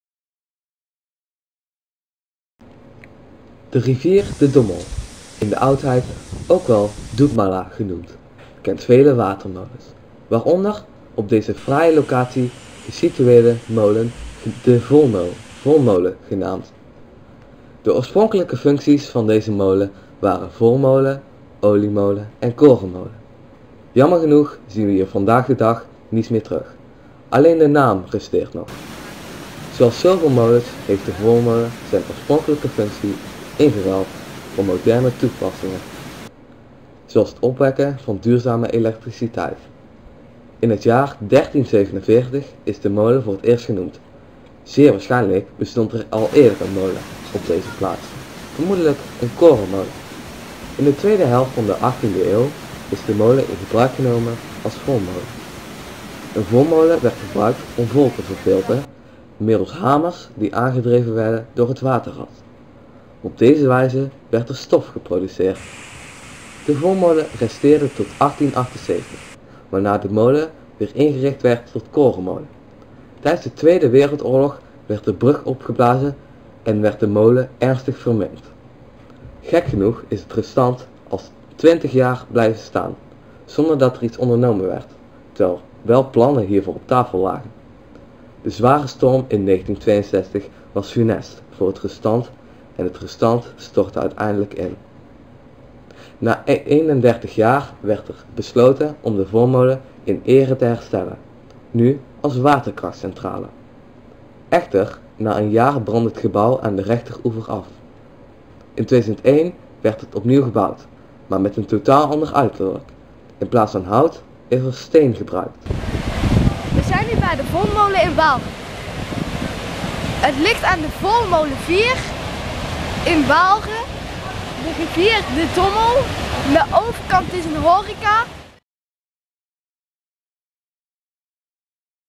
De Volmolen in Waalre
info over de molen en een flashback van 3 jaar terug.